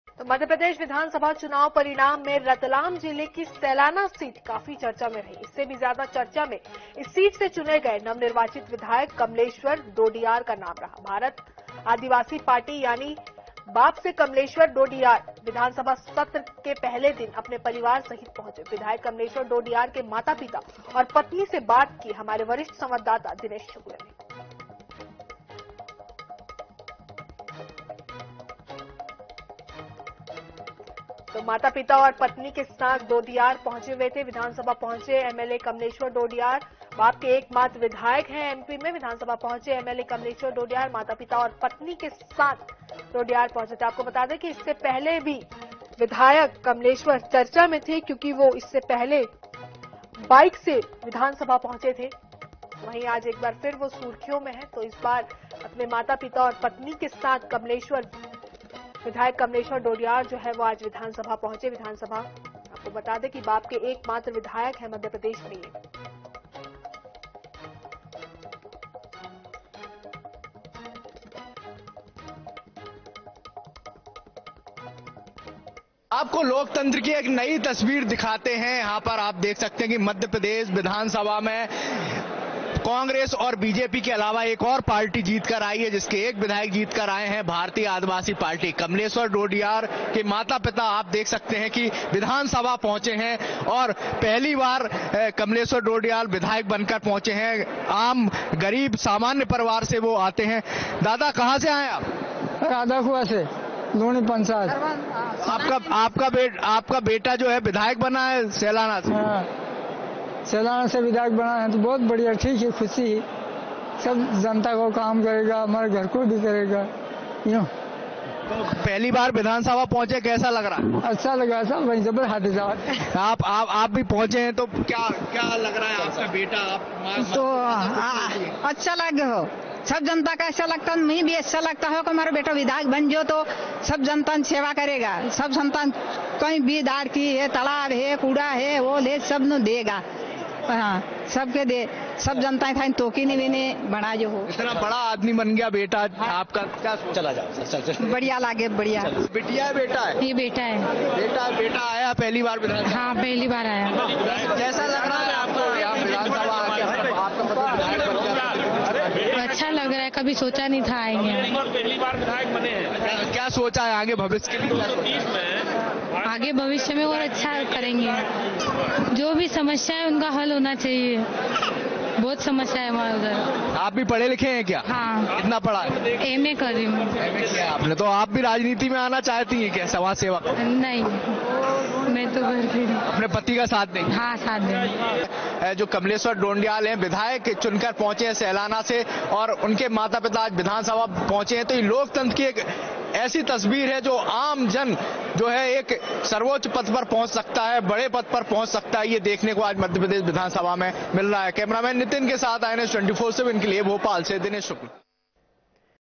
MP Vidhan Sabha Satra: परिवार संग विधानसभा पहुंचे MLA कमलेश्वर डोडियार | Kamleshwar Dodiyar
Incoming Search Terms: